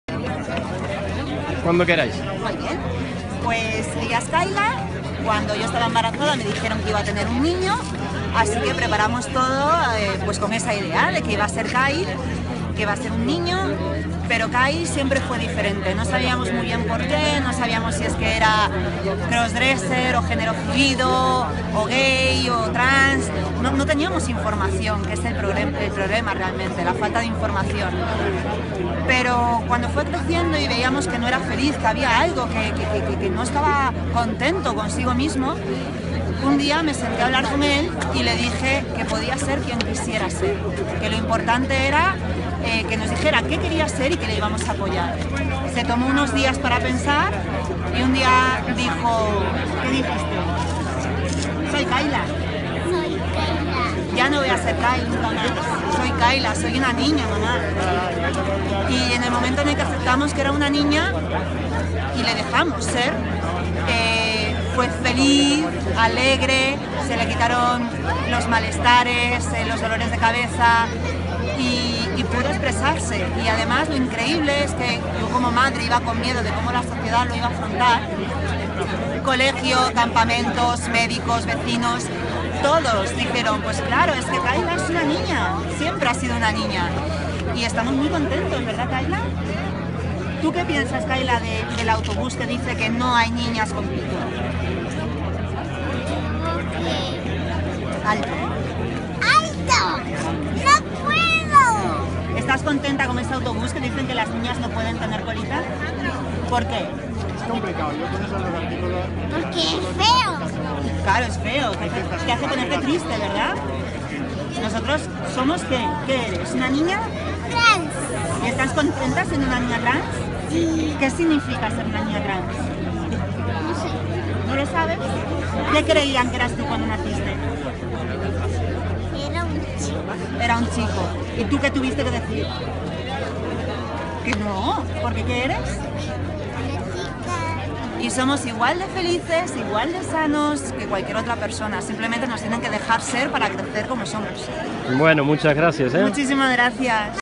La historia de Kayla como símbolo contra la transfobia
En bez.es nos unimos contra la intolerancia y apoyamos a la Fundación Daniela en su concentración en Madrid. Historias como la de Verónica que vive con una hija transexual demuestran una realidad.